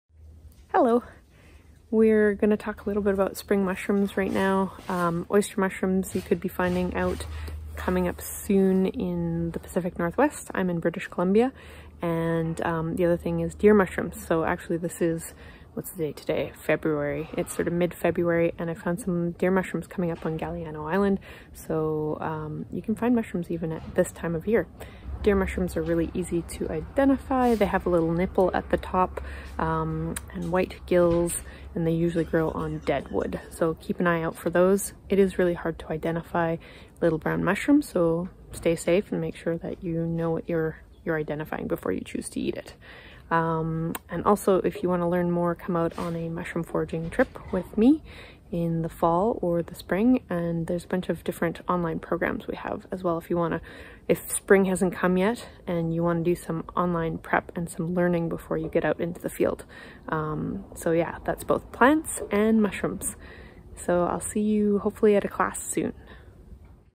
What edible mushrooms can you find in spring? Deer mushrooms
Deer mushrooms are the common name for the Pluteus cervinus complex of mushrooms.  They are very commonly found on the Pacific Northwest coast.
Watch out for poisonous look alikes to the deer mushroom, notably Entoloma species.  So make sure you learn how to identify your poisonous species first!